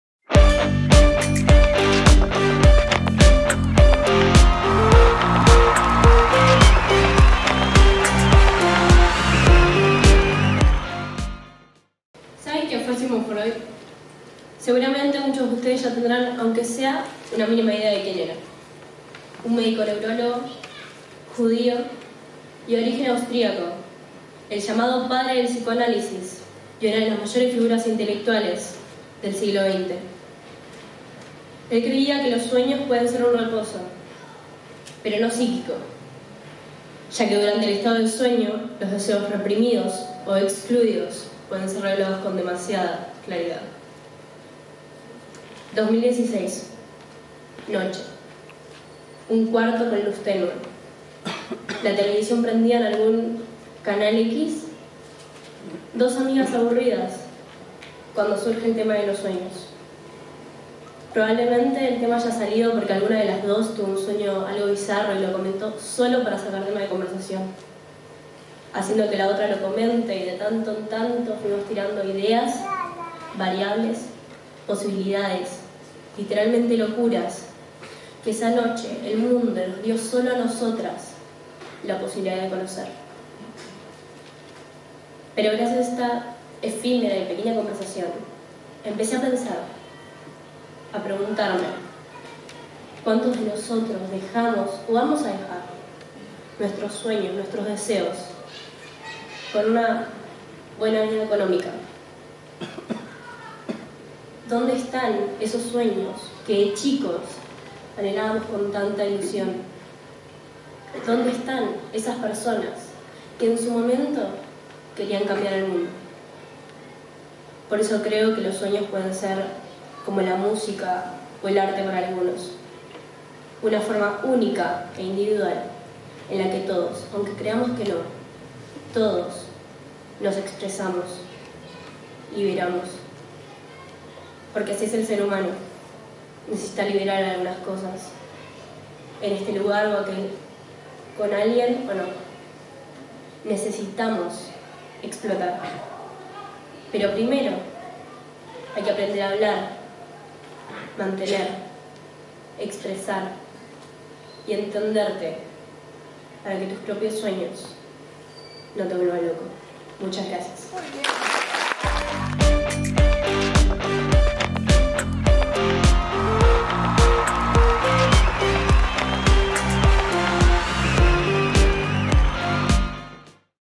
Deseos del inconsciente - Constanza Aiello - Clubes TED-Ed
ClubesTedEd - COLEGIO DEL PARQUE - Licencia: 2019
Titulo: Deseos del inconsciente
Dada por: Constanza Aiello